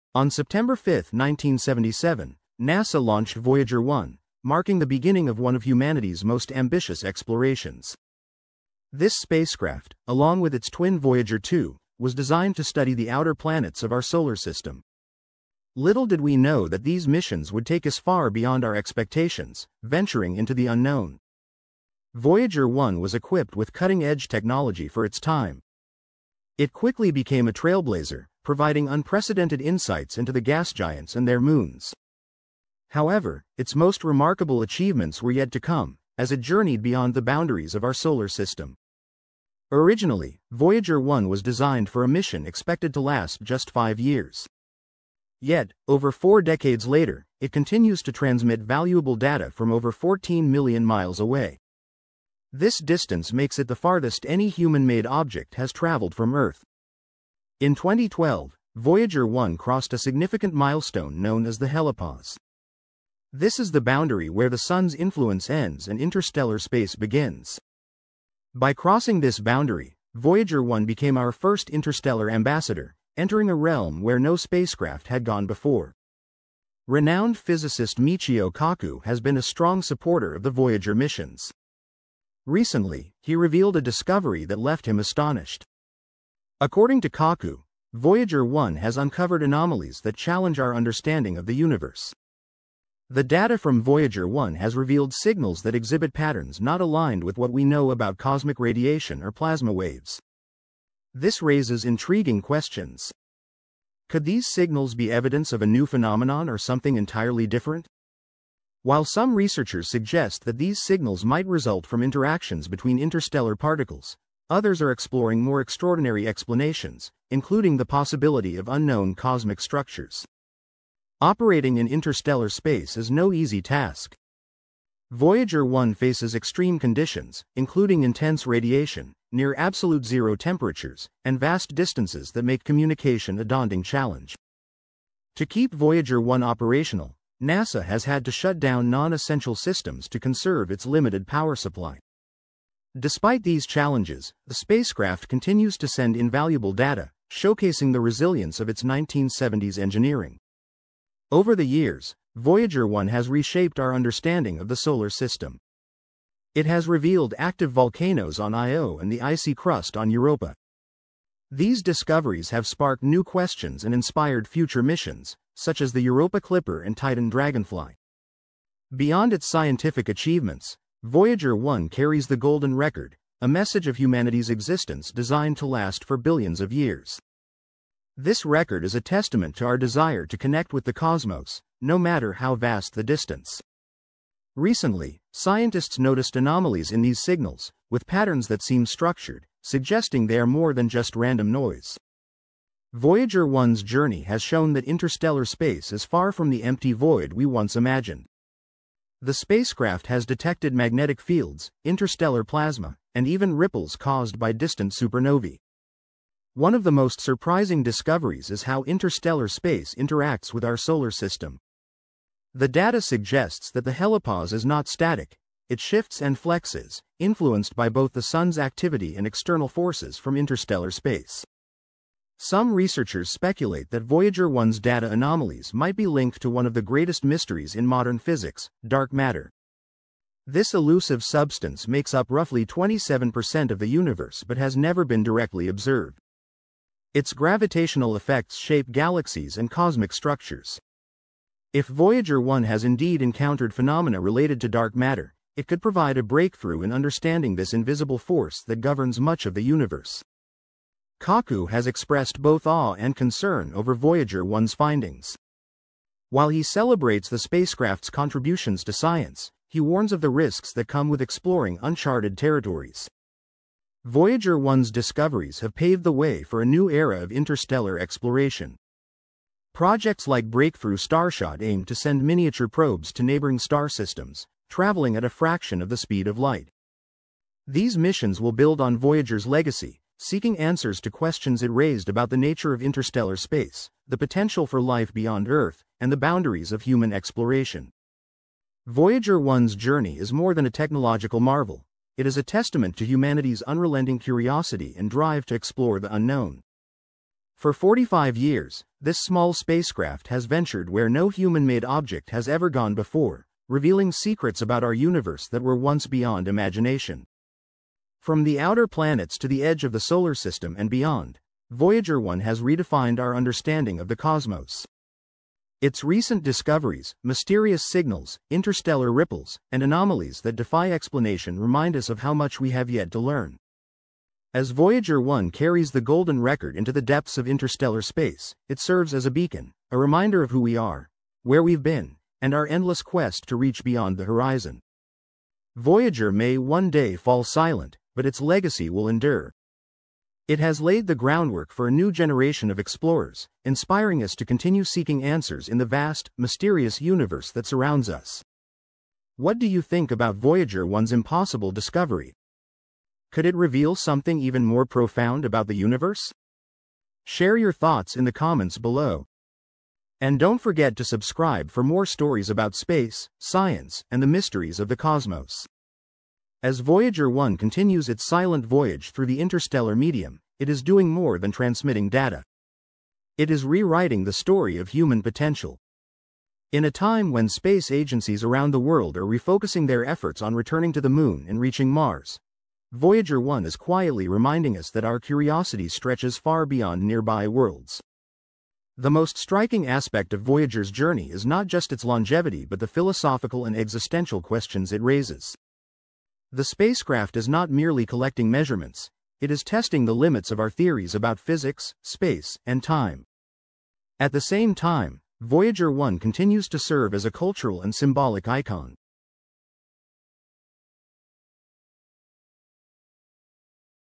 Michio Kaku “Voyager 1 just made an IMPOSSIBLE Discovery after 45 years”
in this video, we explore the amazing journey of NASA's Voyager 1 spacecraft. Launched in 1977, it has traveled farther than any other human-made object, uncovering groundbreaking discoveries in our solar system and beyond. Join us as we discuss its mysterious signals and what they mean for the future of space exploration.